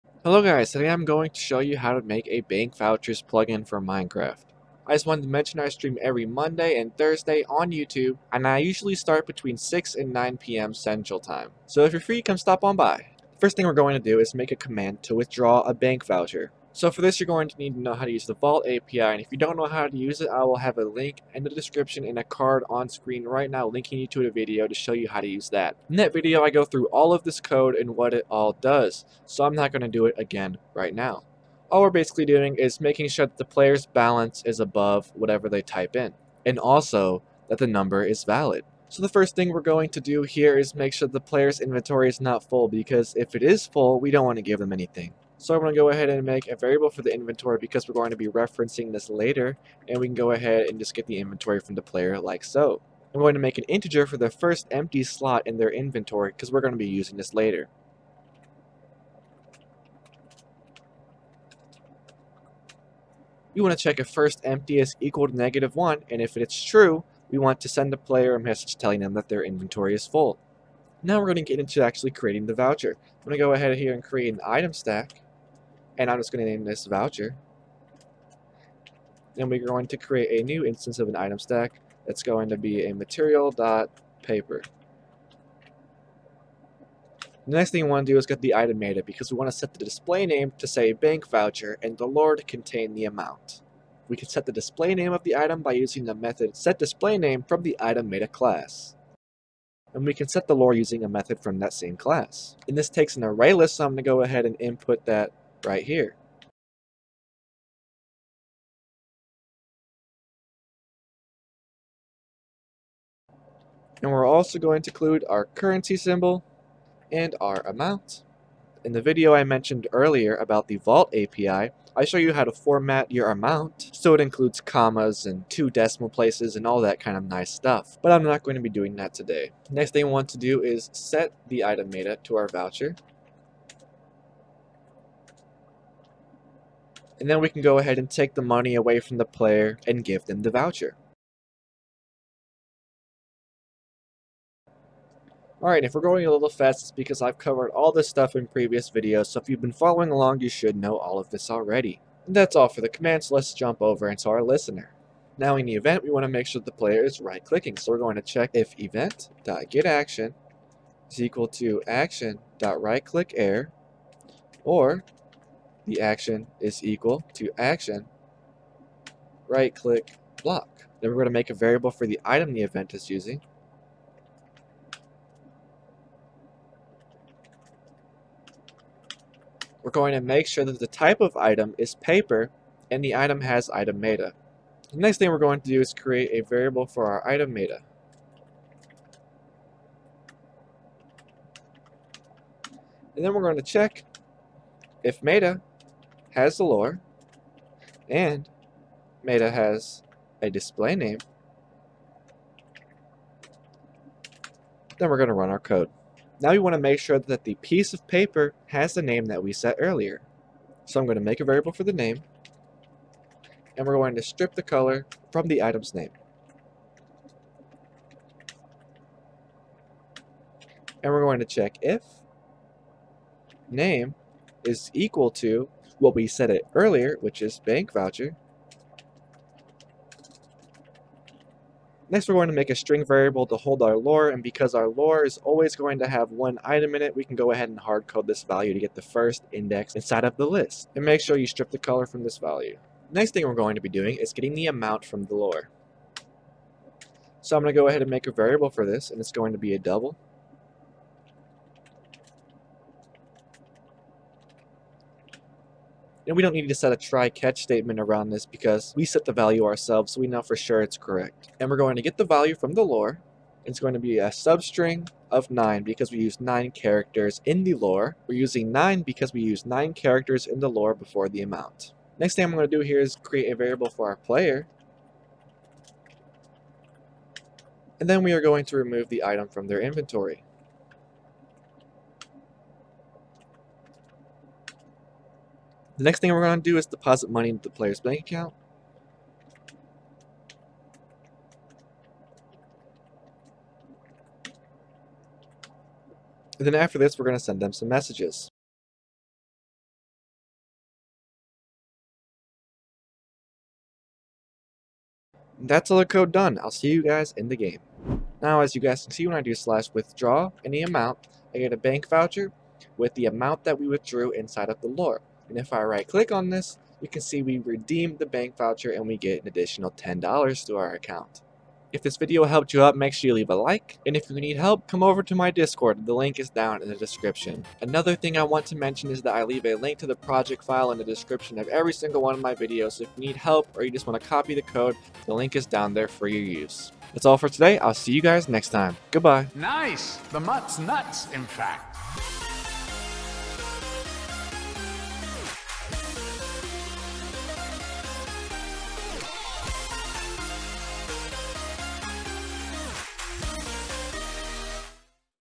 Vouchers / Bank Notes | Bukkit Plugin Tutorial 1.16+
Today we look at how to make vouchers. If you are familiar with the plugin BankNotes its basically that. 99% of the time the answer is one Google search away.
Helpful Links:
How to use the Vault API: